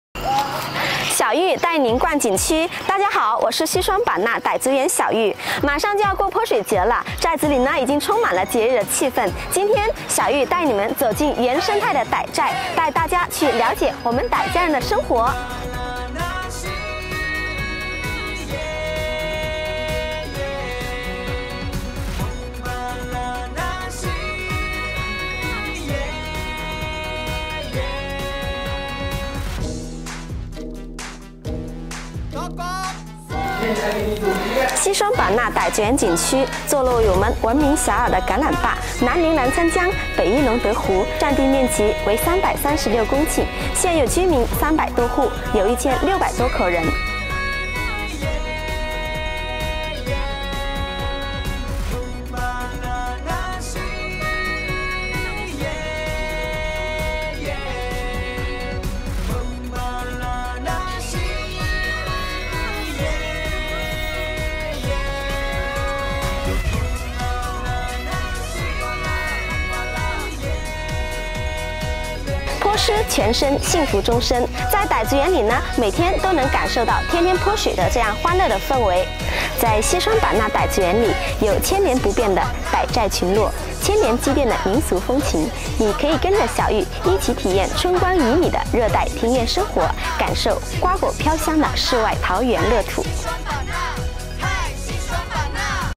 2022云过泼水节｜小玉带你逛景区：走进原生态的傣寨-傣族园【云过泼水节】
“小玉带你逛景区”宣传短片，融入了传统和时尚元素，让“小玉”带领大家云上畅游西双版纳，用科技和新潮的方式展示西双版纳的民俗魅力，以数字科技的全新方式充分体现泼水节的节日氛围及特色活动，将泼水节“狂”“炫”“美”的节日魅力展现出来。